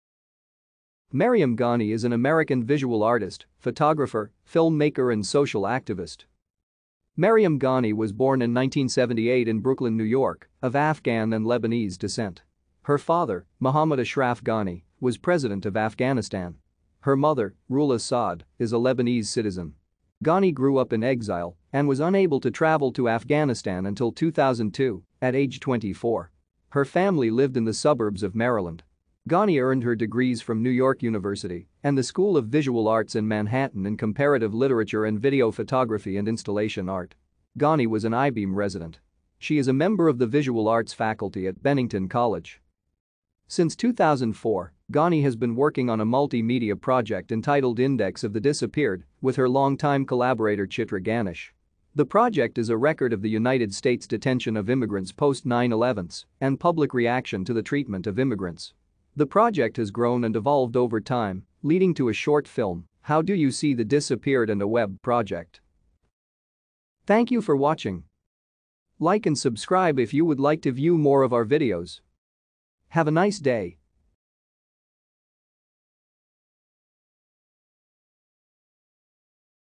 Mariam Ghani
Mariam Ghani is an American visual artist, photographer, maker and social activist.